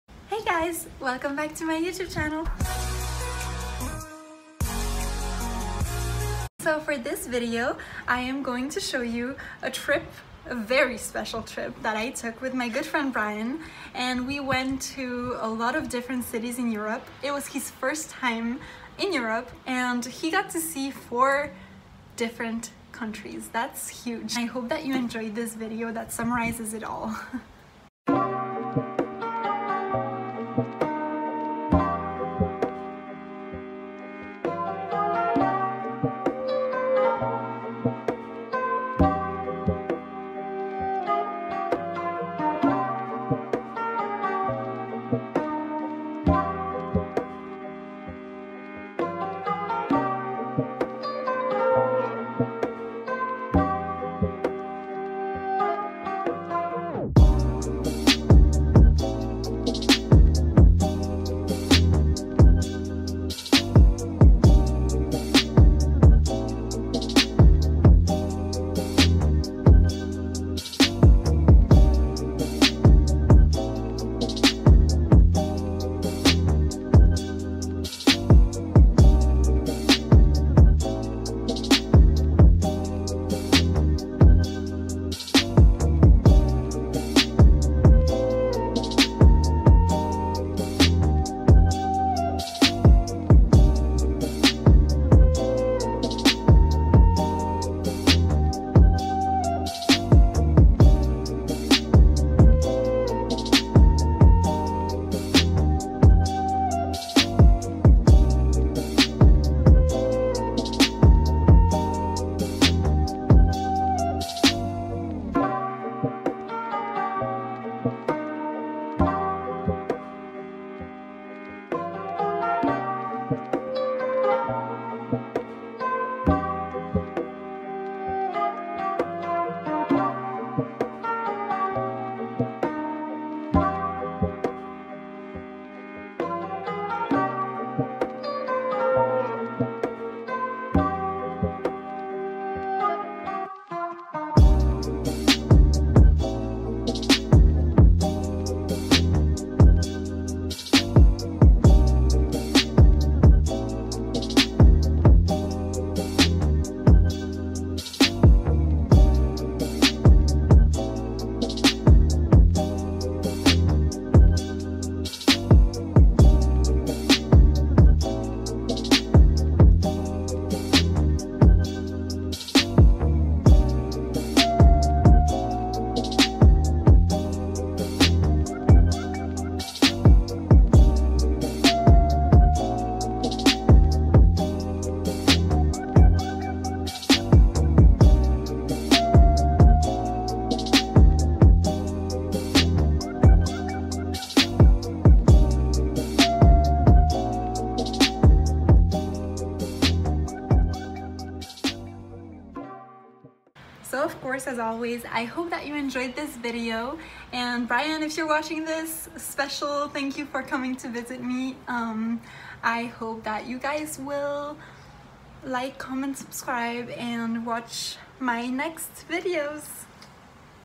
Vlog 10 • A trip through Europe
In this video, I get to show you a very special trip that I took with my good friend Bryan. It was his first trip to Europe so we went to a lot of different cities and he got to see 4 different countries... That's huge!
As always, I hope you enjoy this video and Bryan if you're watching this: special thank you for coming to visit me!